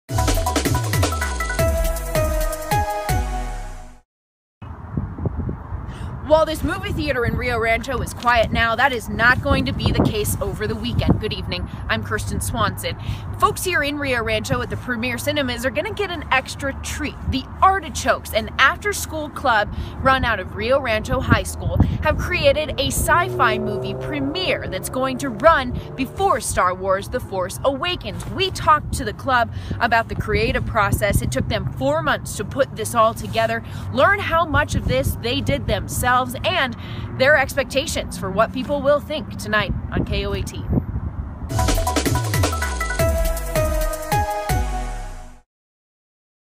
Student film to air before 'Star Wars' screening
A Rio Rancho High School club recently made a sci-fi movie.